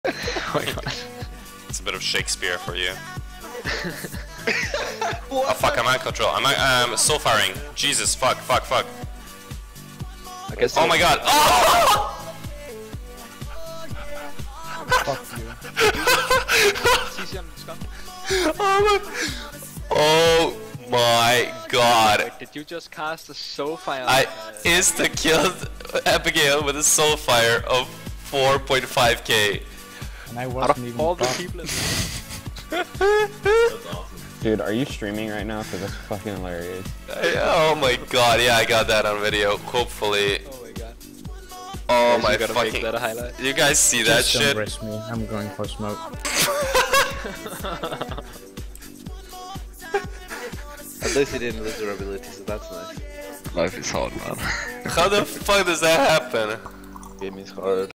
Malconi 1-shot Macro for Warlocks
Priest goes afk after being friendly fired by Malconi -- Watch live